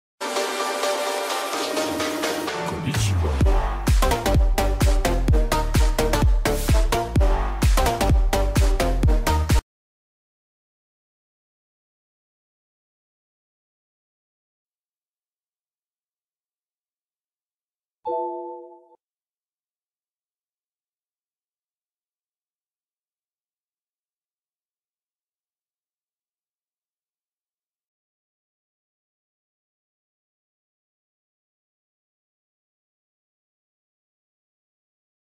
Tutorial: Come monitorare le temperature del proprio pc + download programma
Ecco come monitorare le temperature del proprio pc